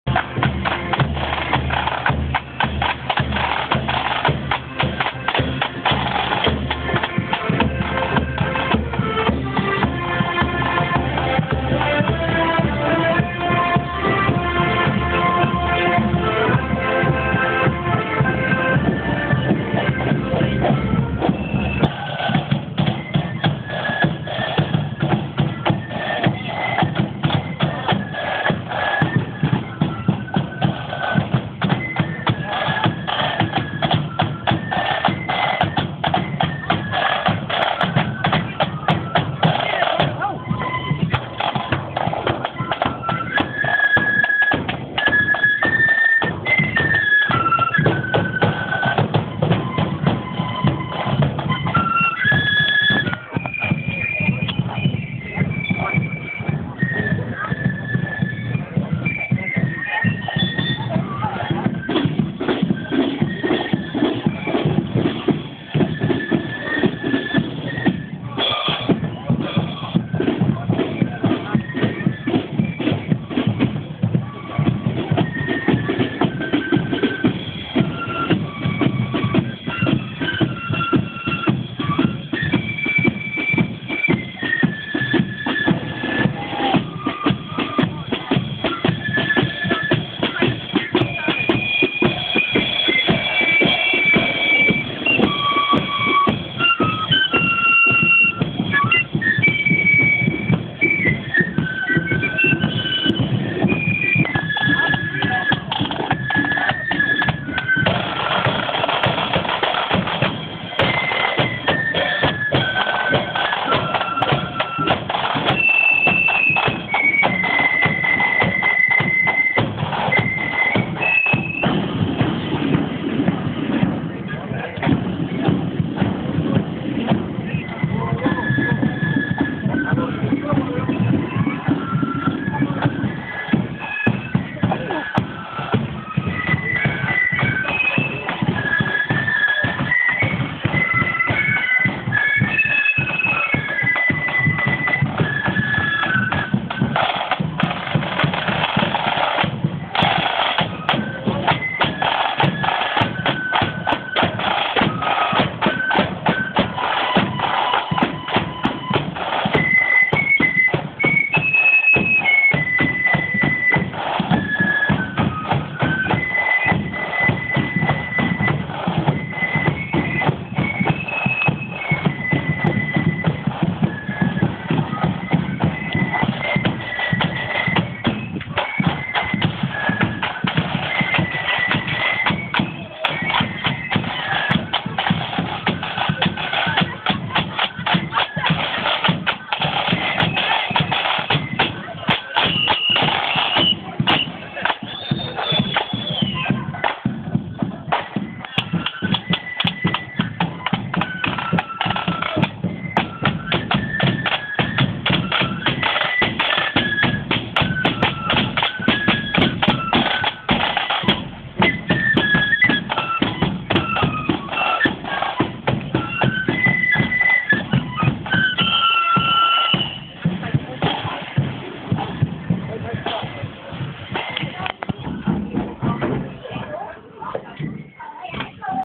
ORANGE WALK LARKHALL
The orange walk walking down Machan road on the 5th of July. Sorry about the video quality. It was filmed on a phone.